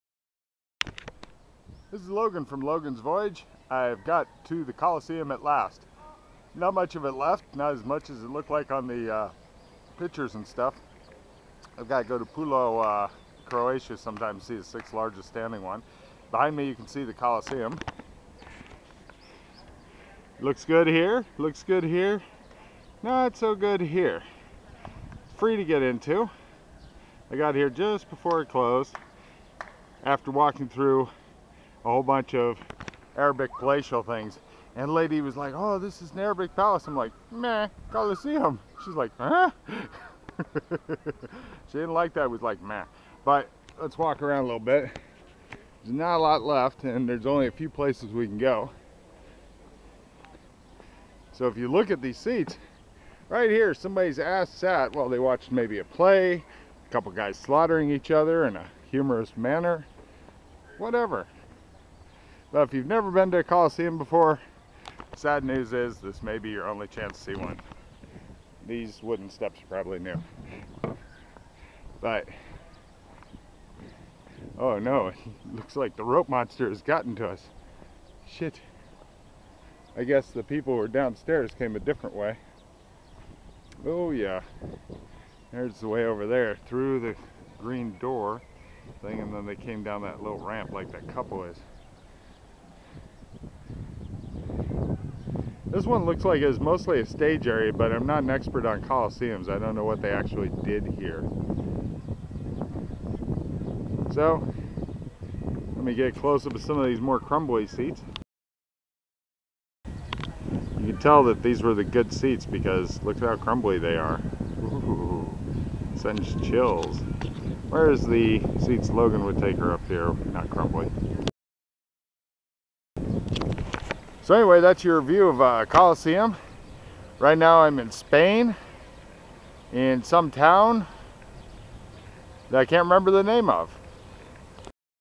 Colleseum
Logan battles late hours and a maze to find what he could have seen for free - next to where he actually went in.